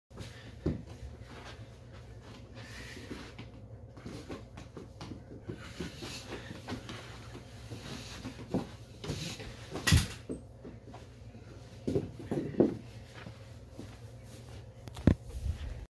Post Shinny Ministick Dangle Snipe | Aug 31 2020
Just some random footage of me in my basement lol

Basement Shenanigans Vol. 65